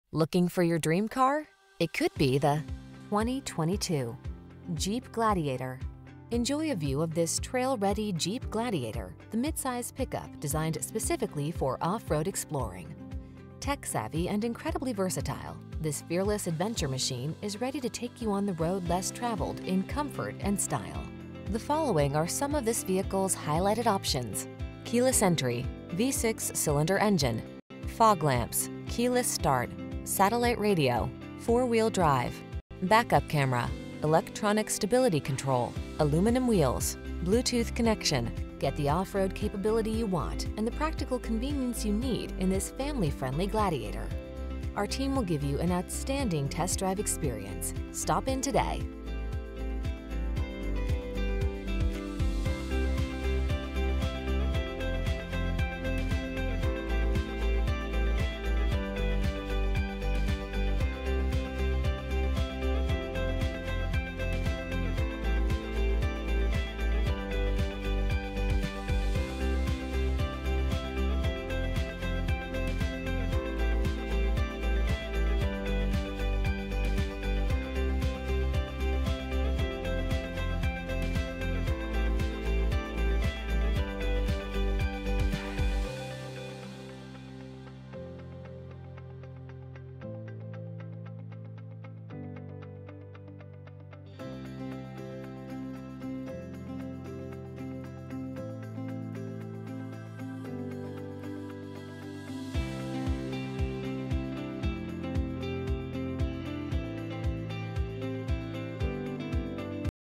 2022 Jeep Gladiator New Smyrna Beach, Port Orange, Daytona Beach, Deltona, Sanford, FL L124661
Snazzberry Pearlcoat New 2022 Jeep Gladiator available in New Smyrna Beach, Florida at New Smyrna Chrysler. Servicing the Port Orange, Daytona Beach, Deltona, Sanford, FL area.